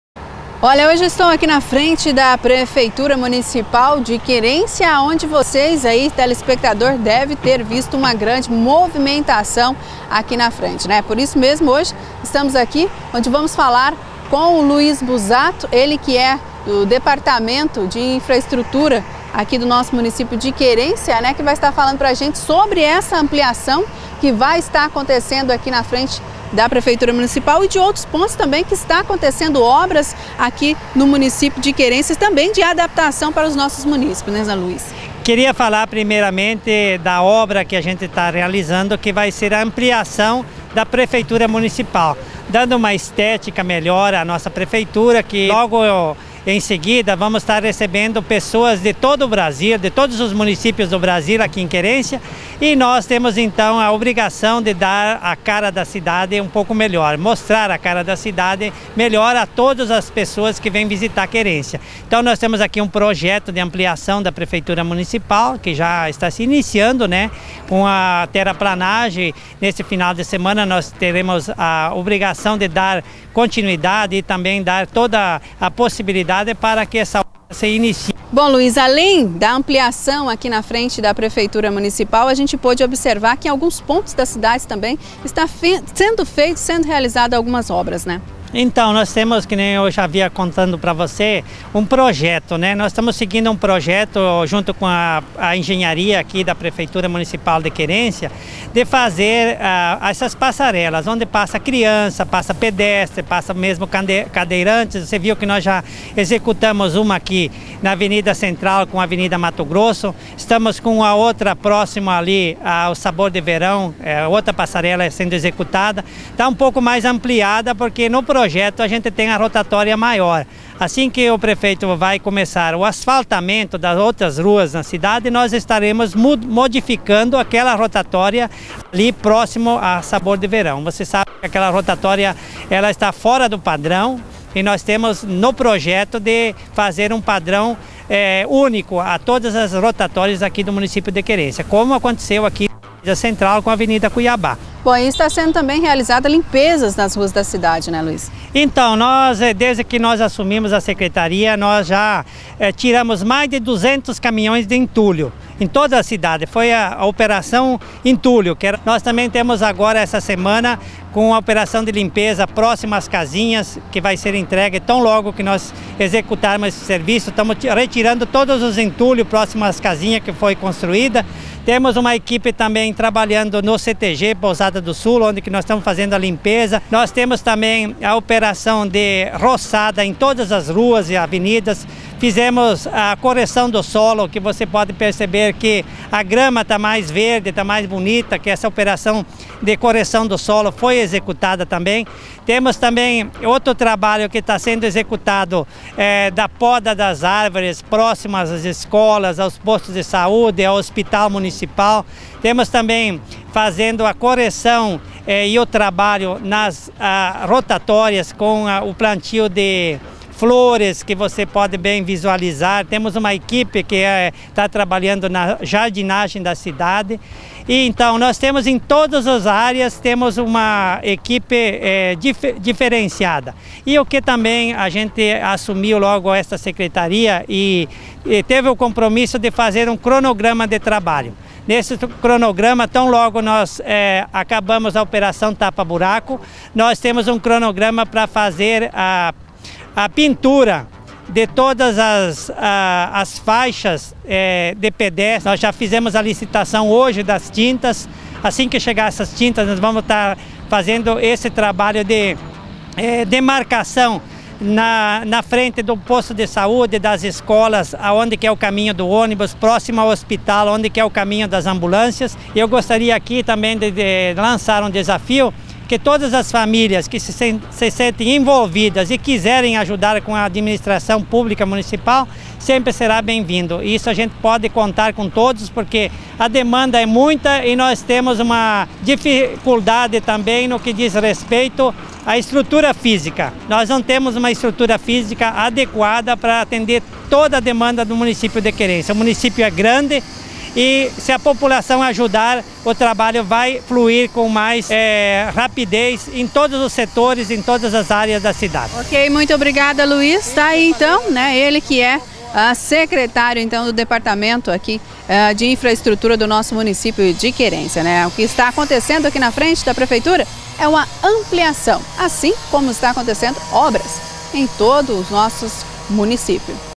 ENTREVISTA COM LUIZ BUSATO SOBRE AS OBRAS NA PREFEITURA E EM TODA A CIDADE
Reportagem: Lu Oliveira
Imagens: Léo Carvalho